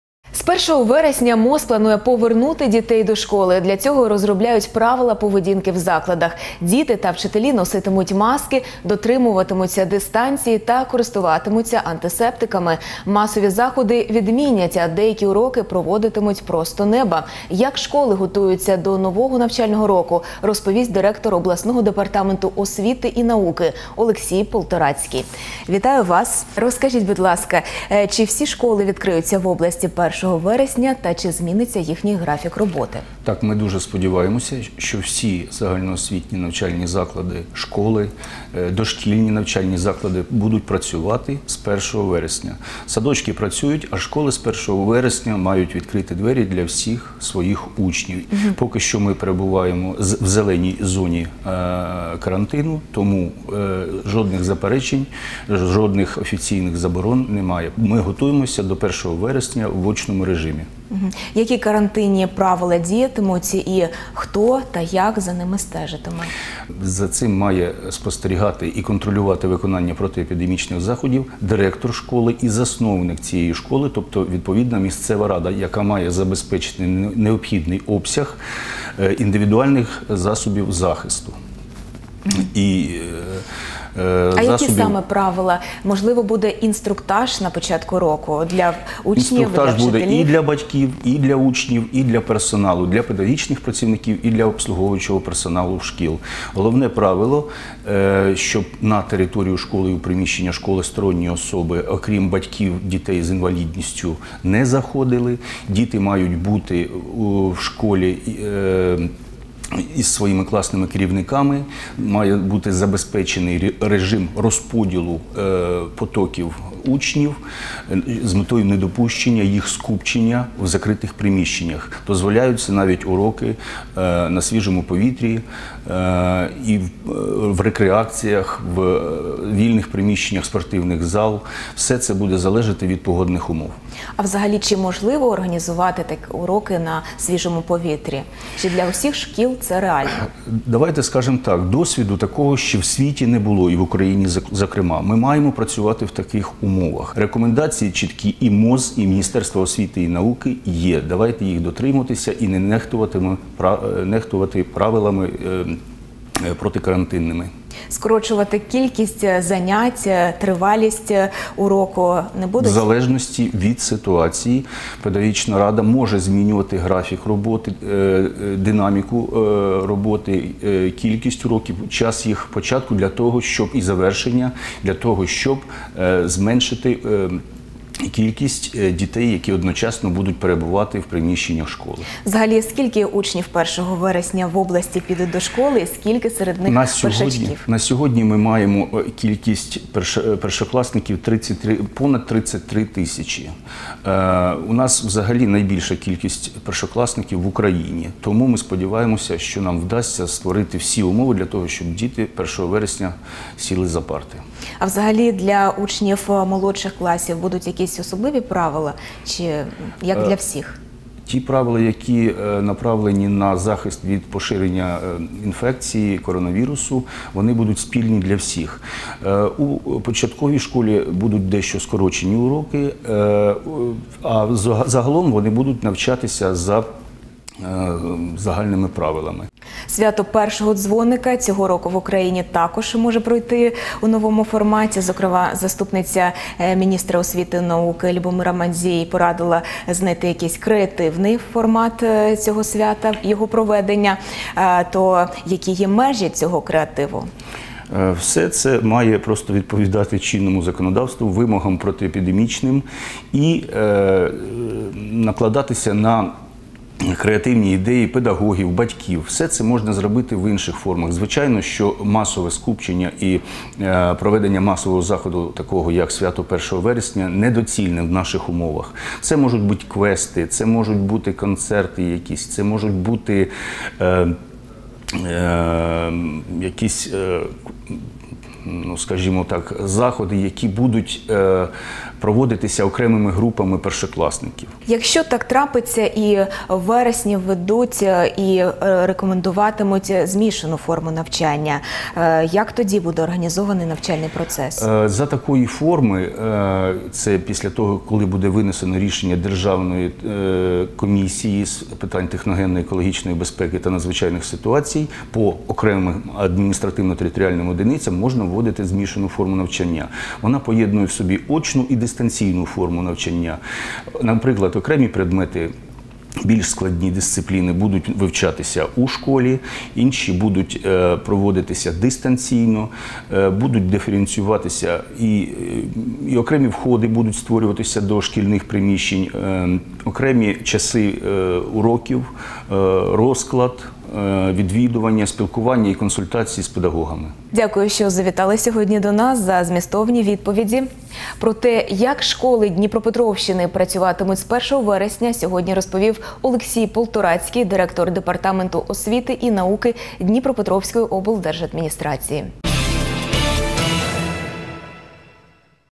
Уроки просто неба та в масках – яким буде новий навчальний рік, розповів Олексій Полторацький
З 1  вересня МОЗ планує повернути дітей до школи. Для цього розробляють правила поведінки в закладах - діти та вчителі носитимуть маски, дотримуватимуться дистанції та користуватимуться антисептиками. Масові заходи скасують, а деякі уроки проводитимуть просто неба. Як школи готуються до нового навчального року, розповість директор обласного департаменту освіти і науки Олексій Полторацький.